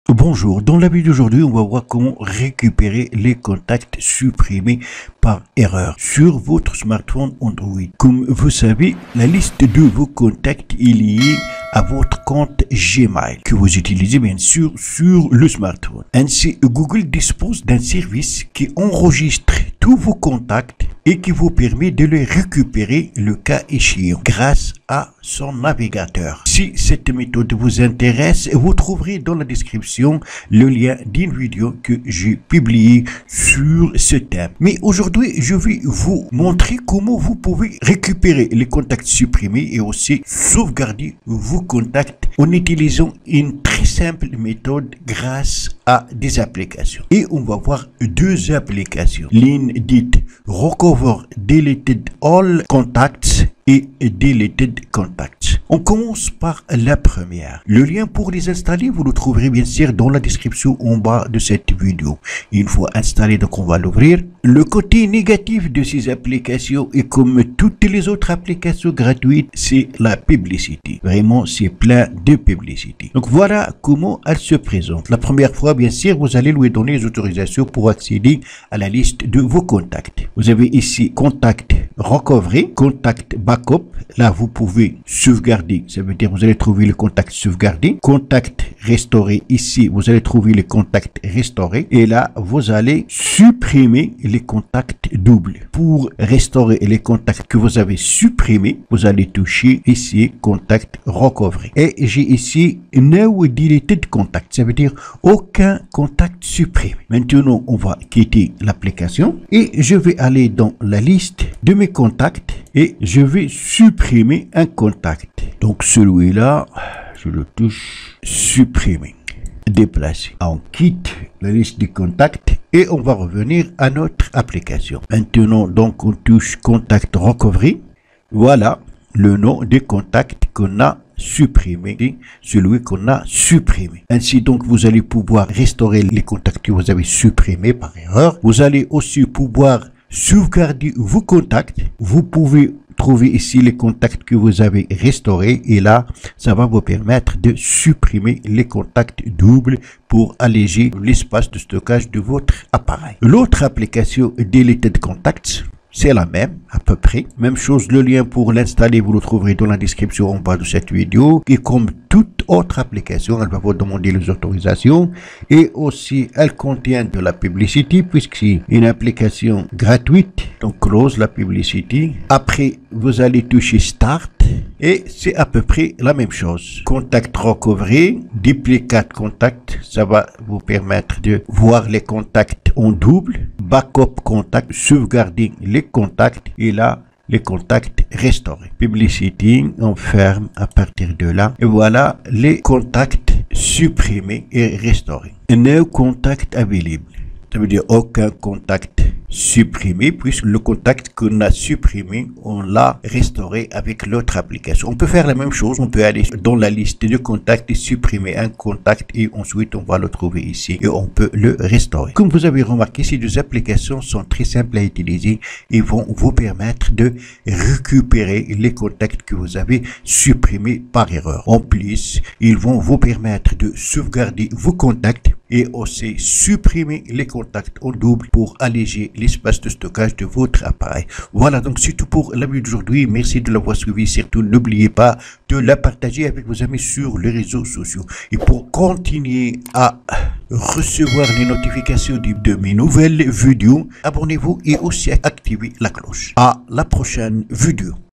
Récupérer facilement les contacts supprimés sur Android à l'aide des applications.
dans cette nouvelle vidéo, je vais vous montrer , comment vous pouvez récupérer les contacts que vous avez supprimés sur Android,  facilement et rapidement grâce des applications .